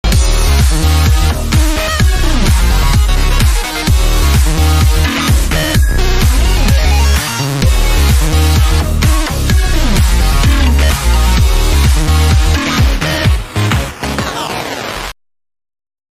(fnaf sfm) Collab part 21 for SorooshKing
part 21 for his Mutant collab. Proud with this one too. Although i was trying out a new style with some lighting and shot composition and I think it came out nicely for the final part of the collab.